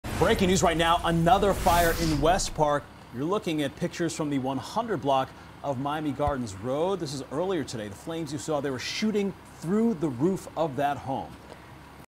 Fire erupts at home in West Park
A fire erupted Thursday morning at a home in West Park.